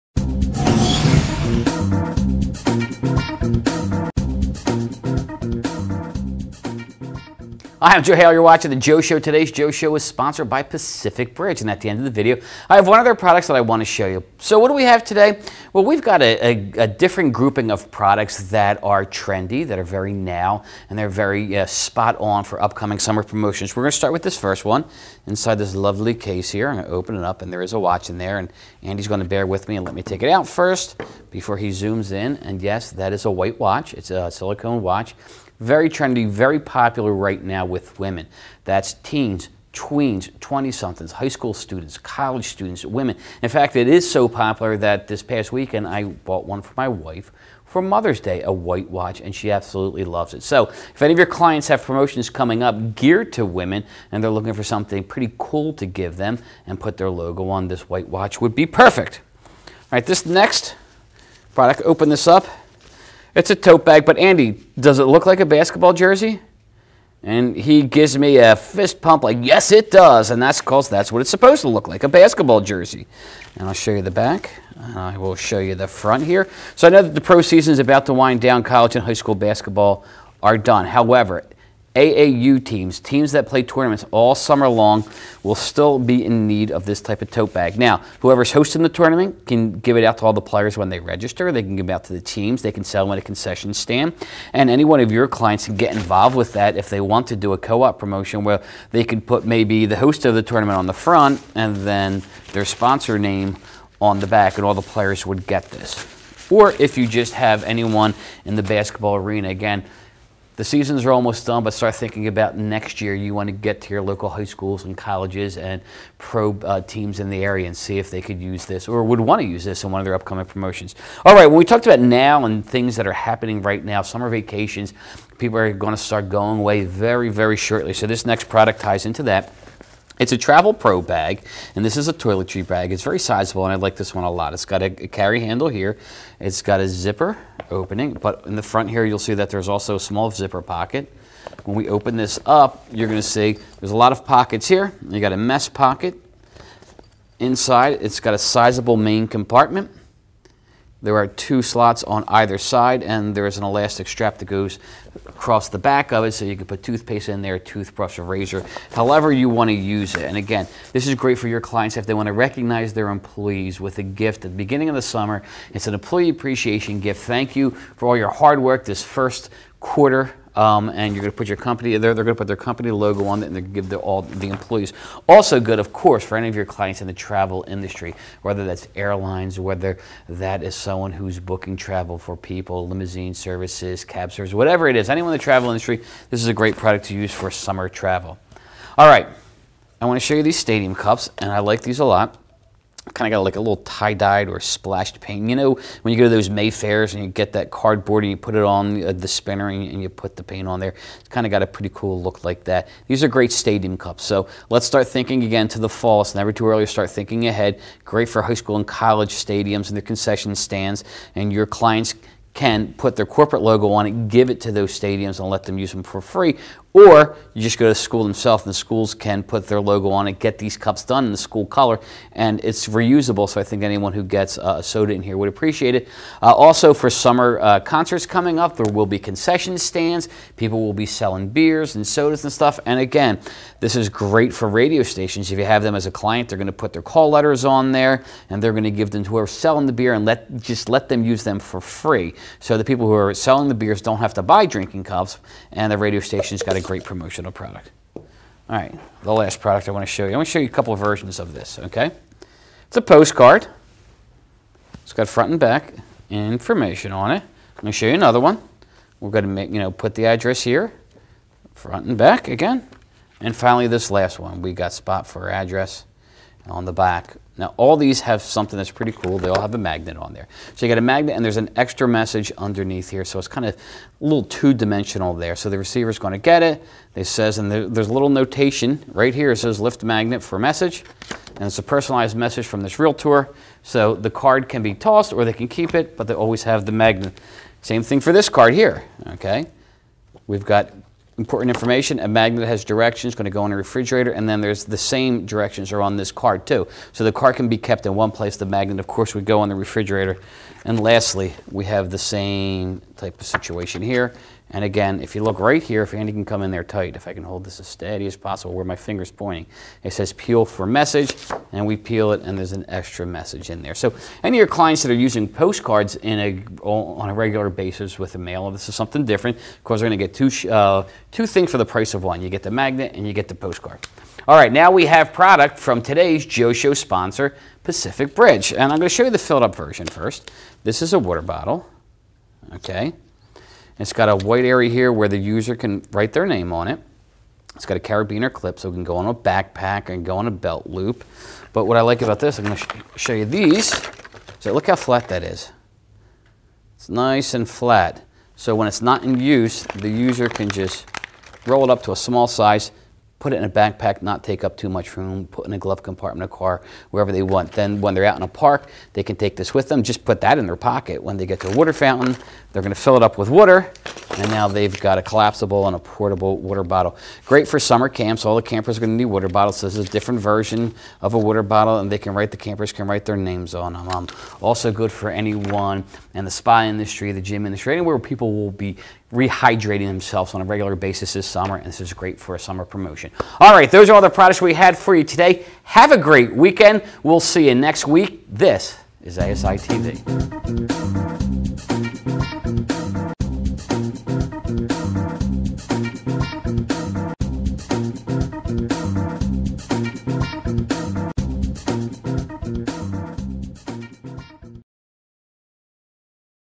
Trendy Products - The Joe Show
In this episode of The Joe Show, Managing Editor Joe Haley shows off some cool products that are timely for upcoming promotions. Need a trendy item for a promotion targeted to women? Looking for a completely unique product perfect for outdoor summer campaigns? Check out this episode.